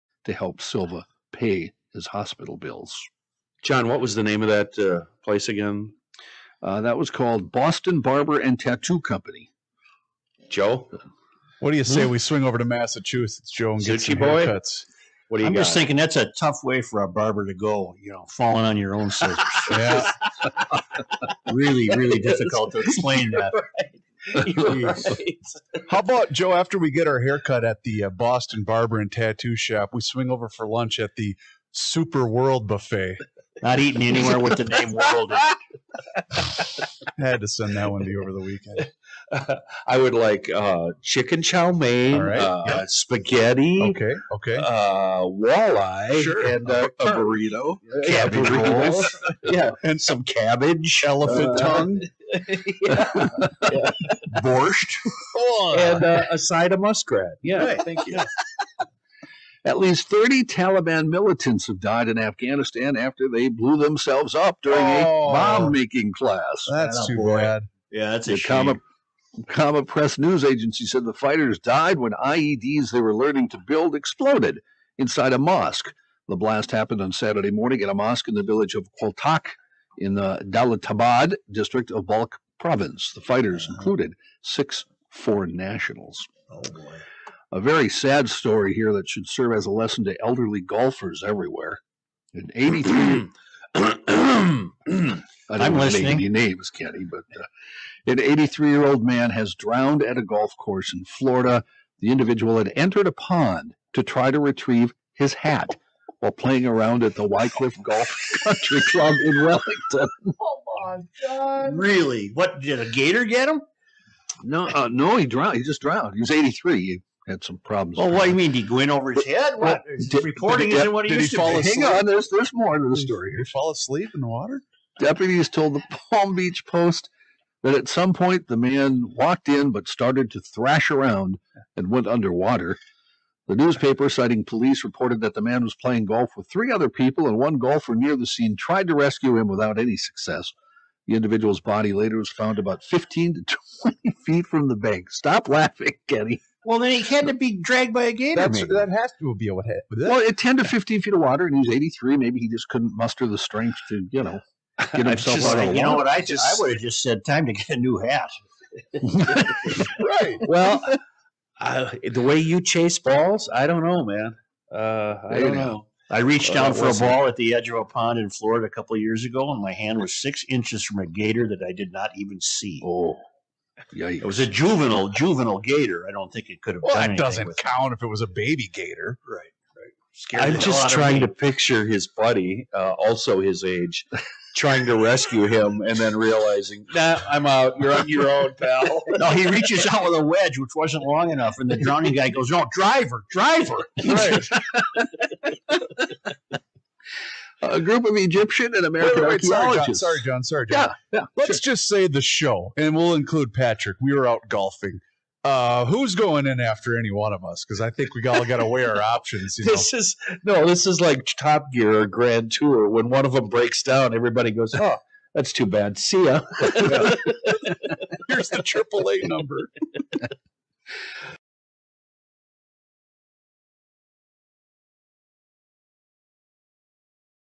Guitar News with John Heidt included a couple of unfortunate mishap stories
For full episodes of the Garage Logic Podcast, visit GarageLogic.com!